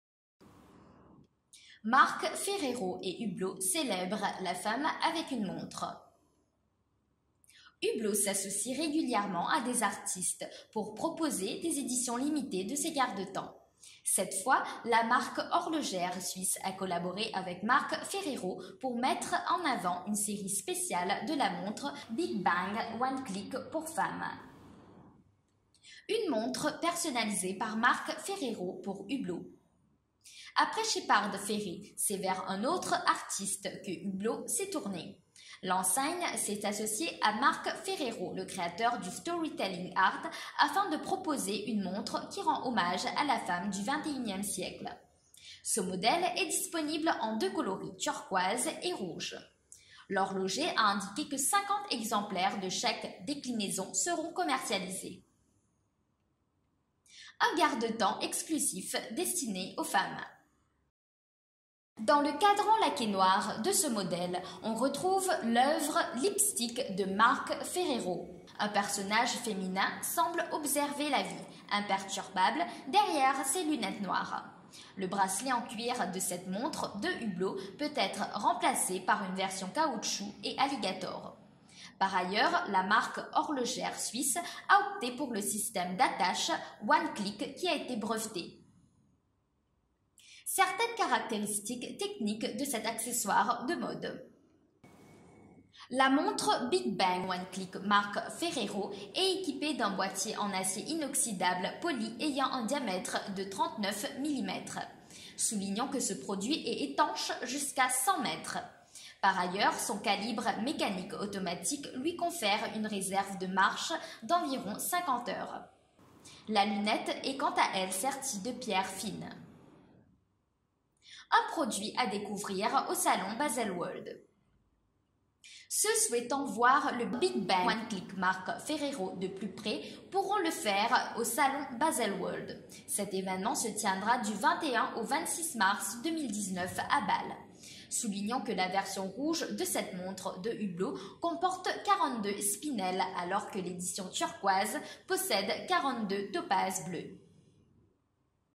Montre, Hublot collabore avec Marc Ferrero afin de proposer un accessoire pour femmes
Hublot s’associe régulièrement à des artistes pour proposer des éditions limitées de ses garde-temps. Cette fois, la marque horlogère suisse a collaboré avec Marc Ferrero pour mettre en avant une série spéciale de la montre Big Bang One Click pour femmes.